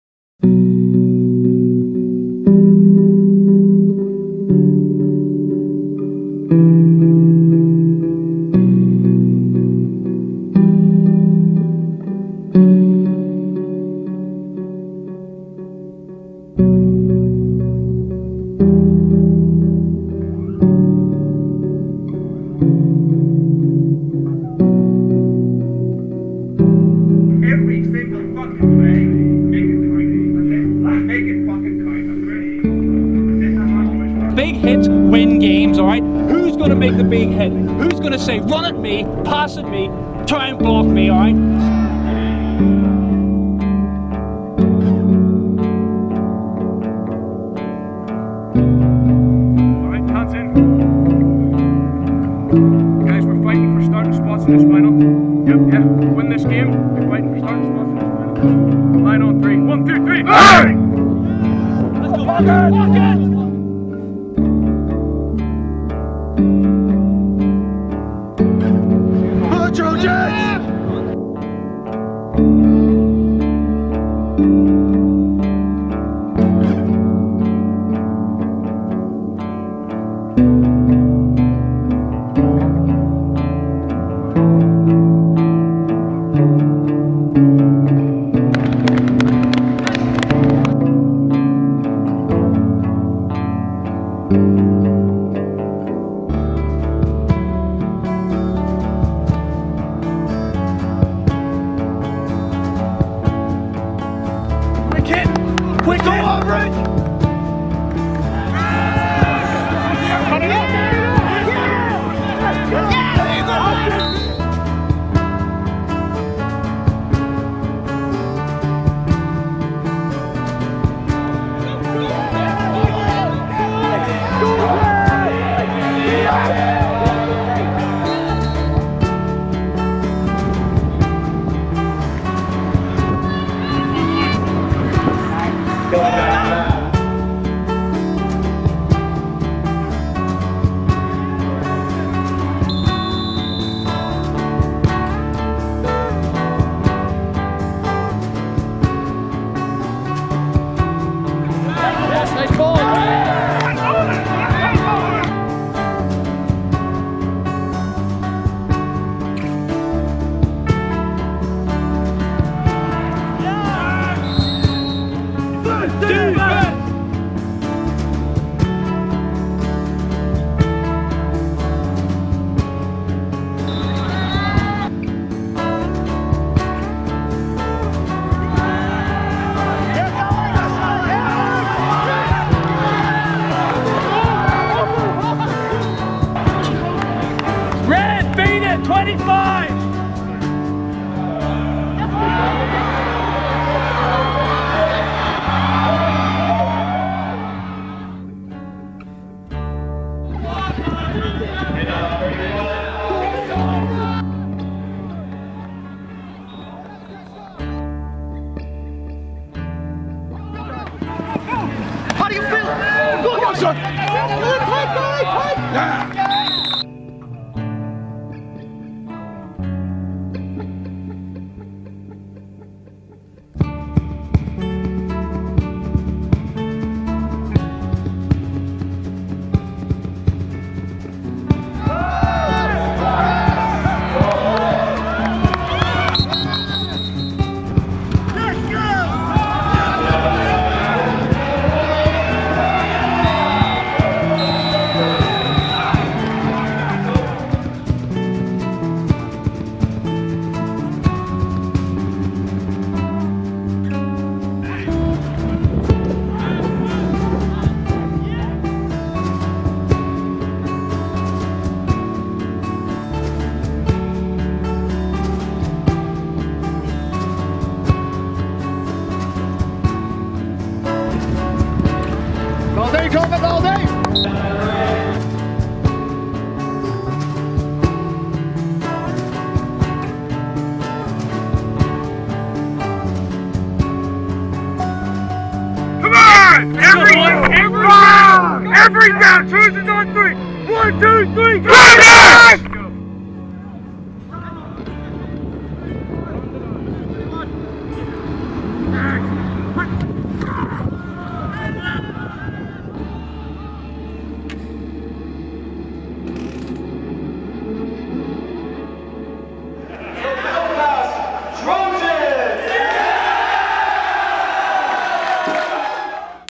Atlantic Cup 2013 Belfast Trojans
The Belfast Trojans are Atlantic Cup Champions after two impressive victories against the Amersfoot Untouchables and Trinity College Dublin.